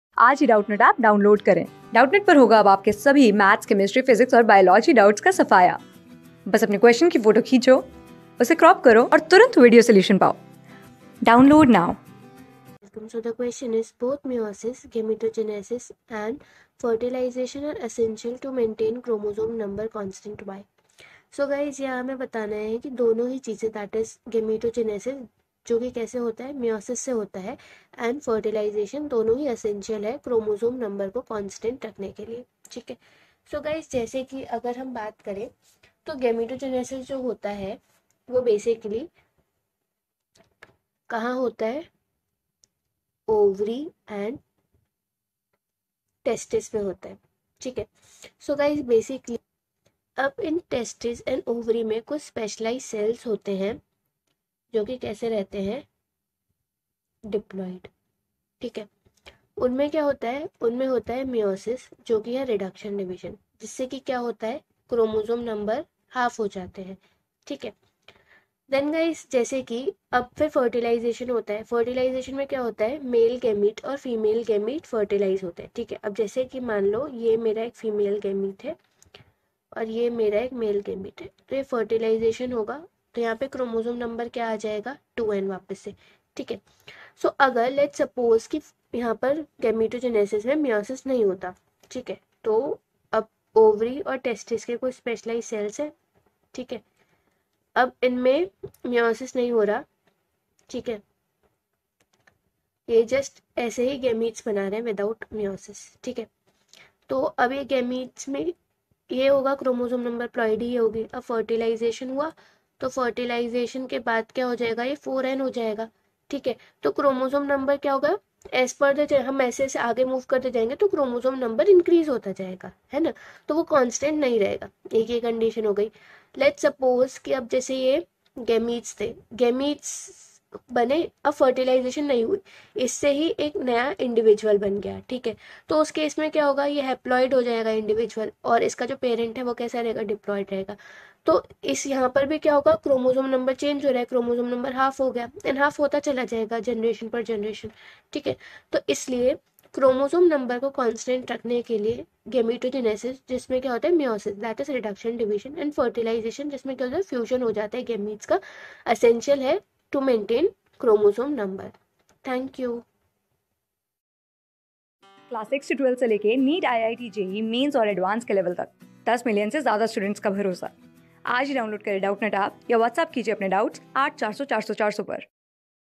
Both meiosis (gametogenesis) and fertilization are essential to maintain chromosome number cons...
Both meiosis (gametogenesis) and fertilization are essential to maintain chromosome number constant. Why?
Class: 12
Subject: BIOLOGY
Chapter: REPRODUCTION IN ORGANISMS
Board:CBSE

👉 Have Any Query? Ask Us.